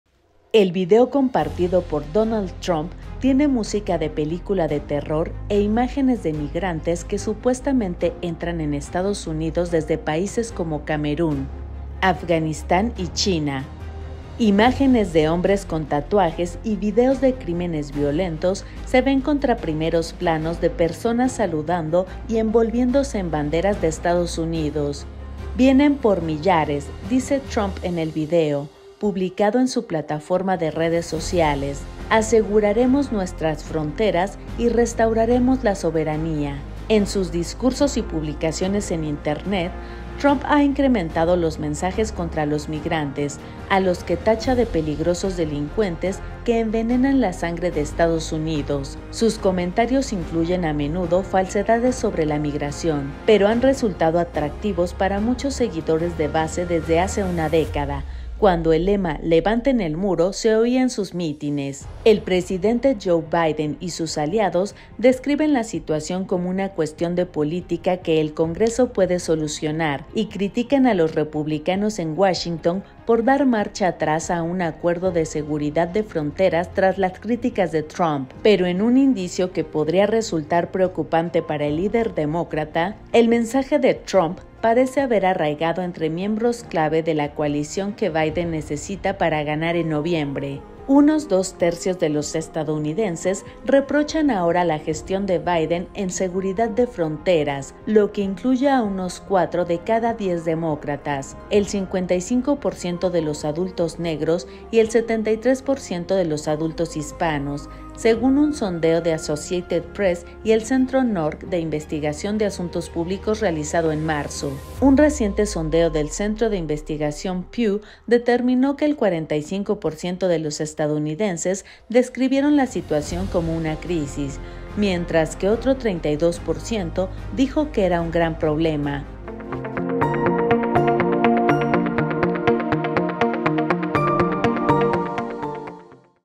Trump incendia con el miedo a los migrantes. Y está ganando votos, incluso demócratas
El expresidente describe las detenciones récord registradas recientemente en la frontera suroeste como una “invasión” orquestada por los demócratas para transformar Estados Unidos.